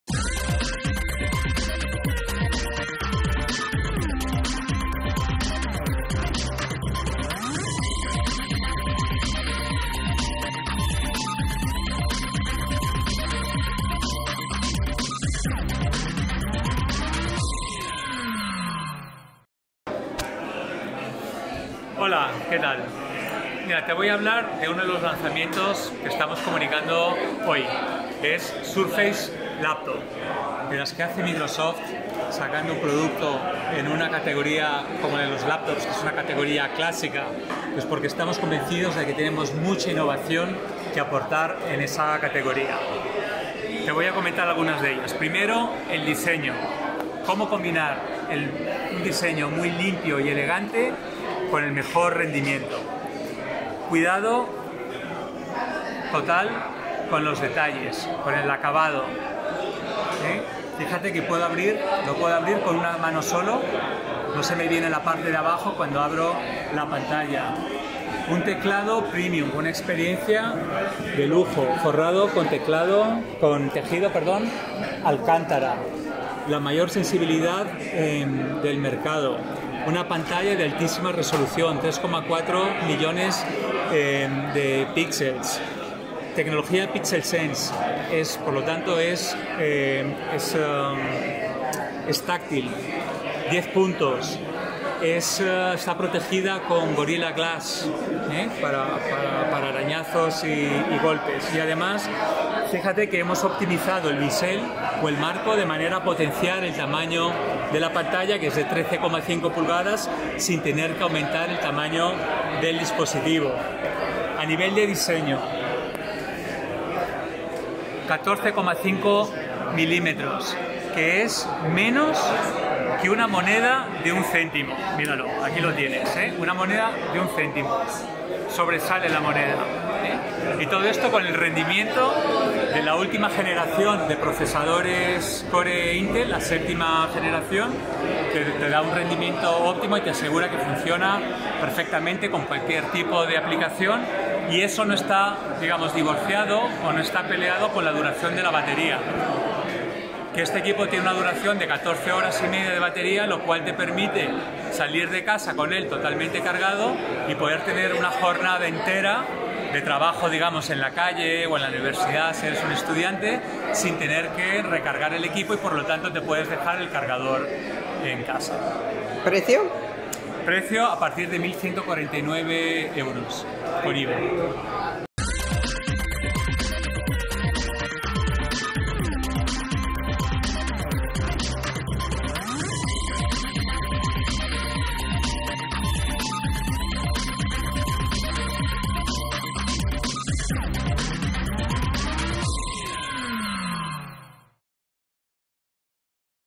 Surface laptop: innovacion y rendimiento
Surface Laptop, tiene un peso de 1,22 g y un grosor menor a las monedas de un céntimo (14,7 mm), que incluye no sólo la última familia de procesadores de Intel (i7), sino batería con 14 horas de uso. La pantalla es táctil, de 13,5 pulgadas, ‘pencil sense’ para utilizar el lápiz.